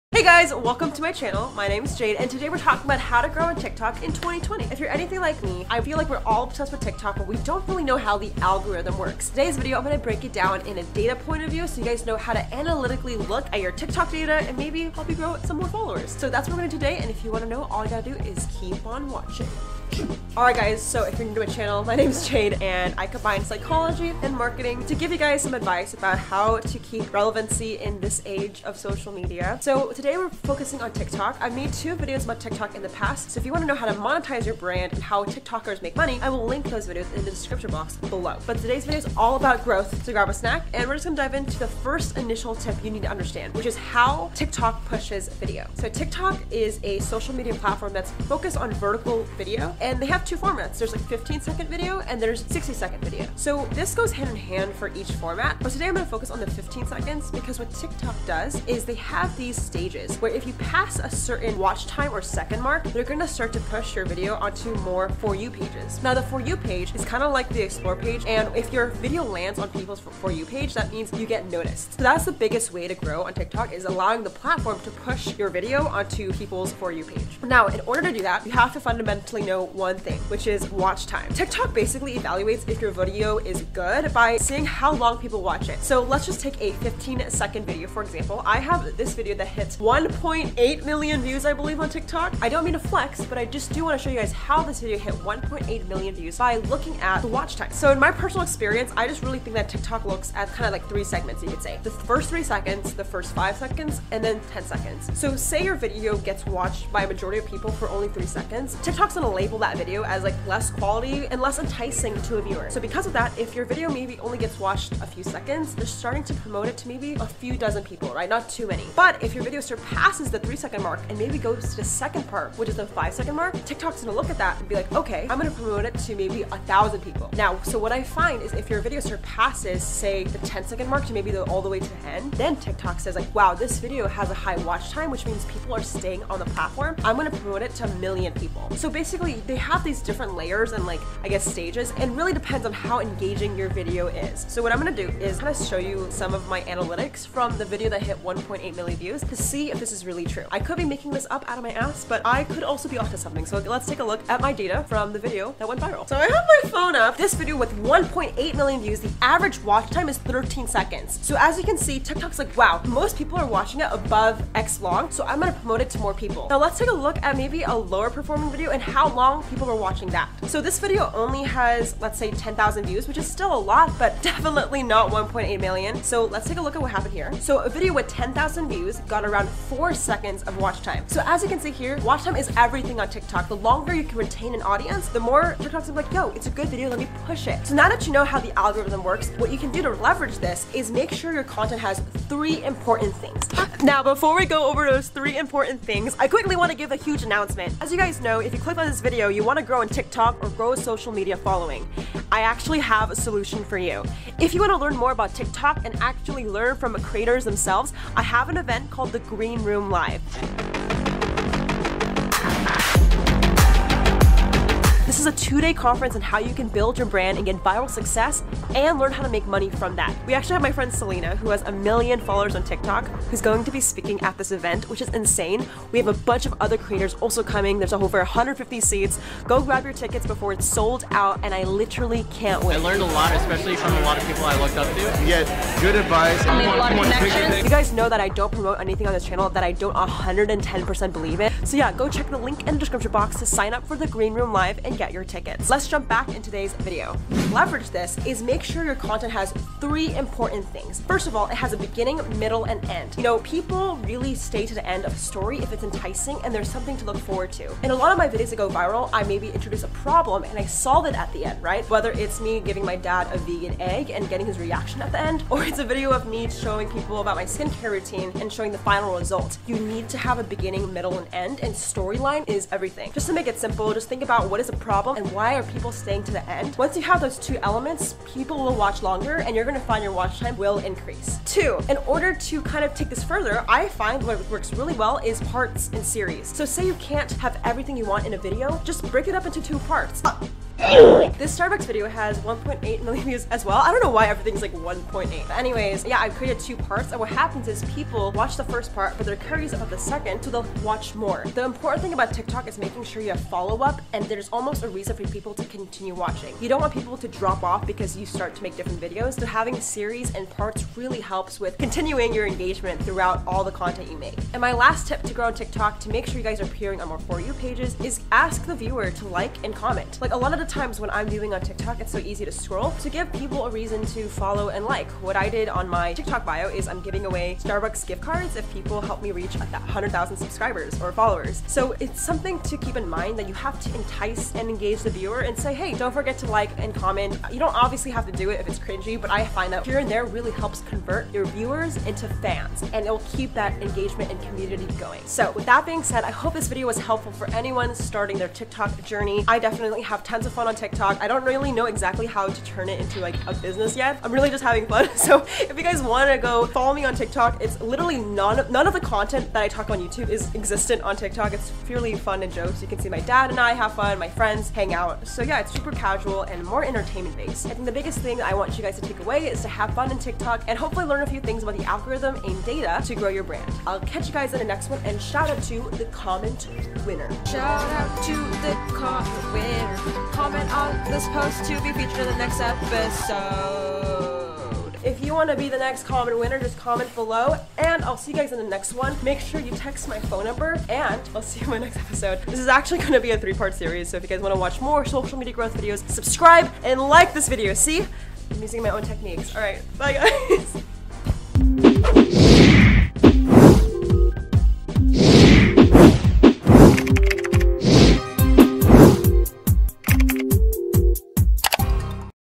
TikTok Algorithm: How To Go VIRAL In 24 Hours
Today we talk about how to GROW on tiktok! We go over all the algorithm hacks!